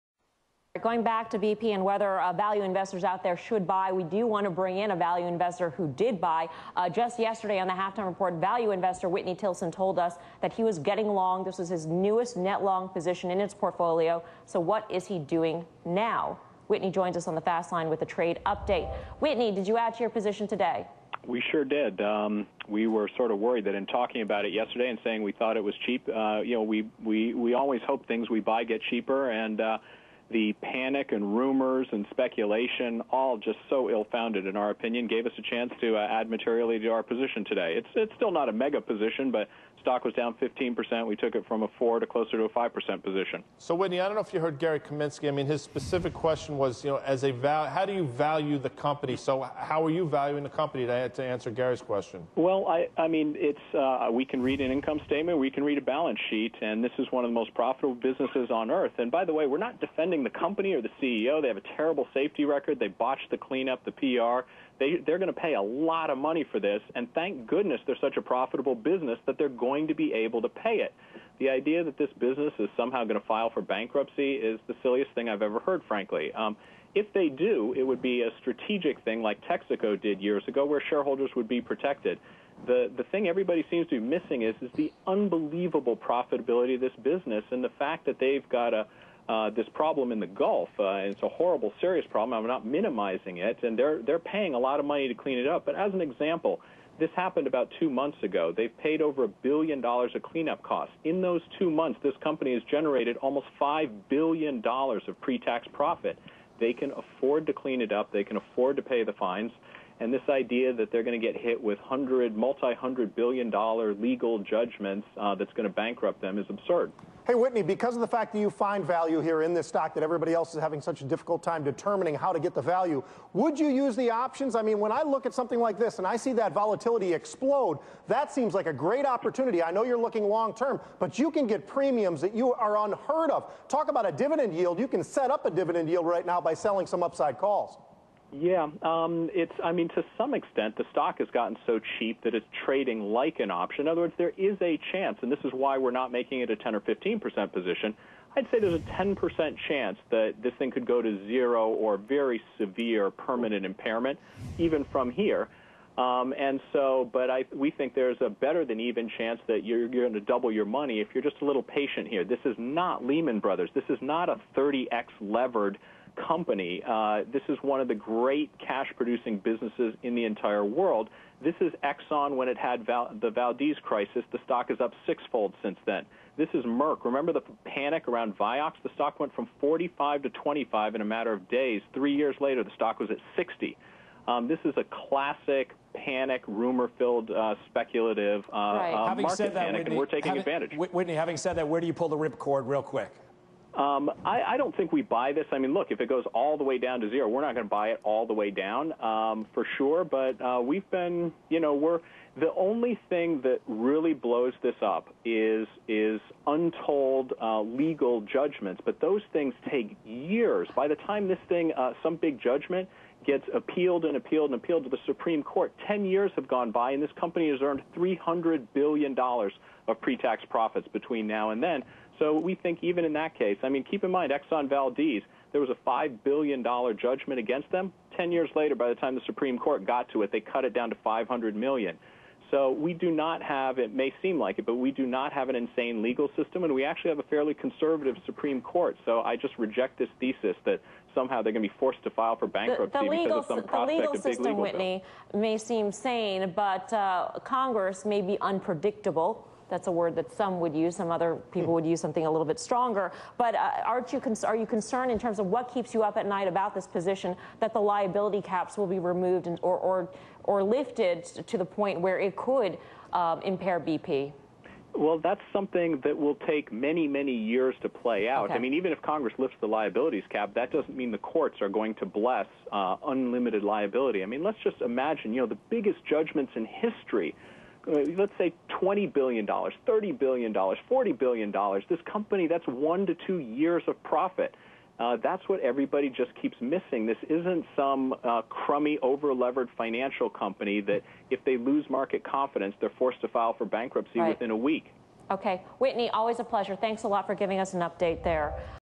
Whitney Tilson on Why He's Buying BP
Whitney Tilson on Why He's Buying BP, 6/9/10